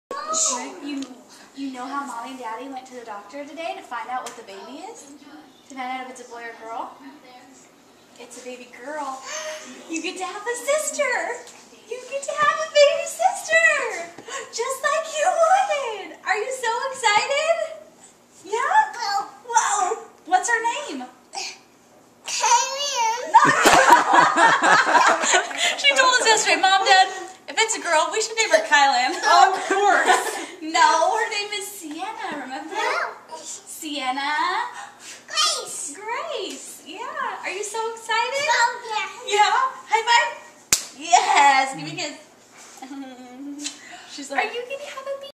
It's a Girl!
Telling Allie she is going to have a little sister!